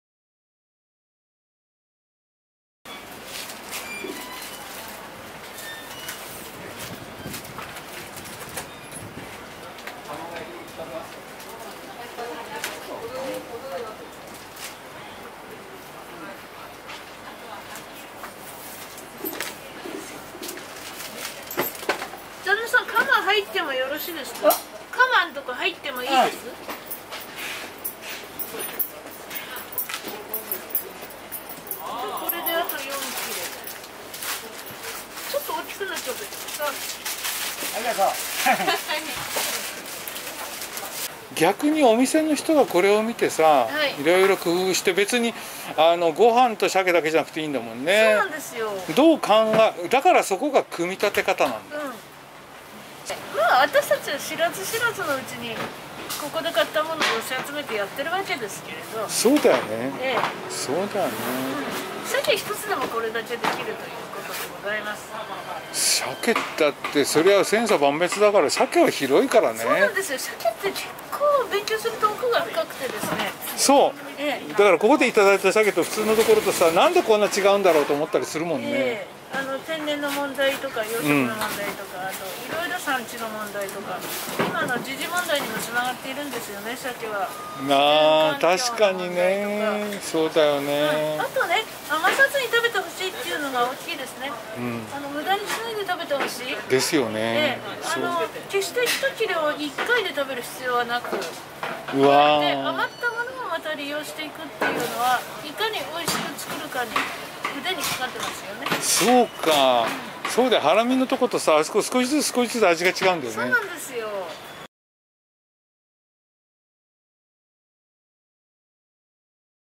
鮭ひと切れから 拡がる和の料理
「鮭とごはんの組み立て方」(誠文堂新光社)
佐藤友美子著
築地のしゃけこ　さん
2021年6月1日　築地中通り　昭和食品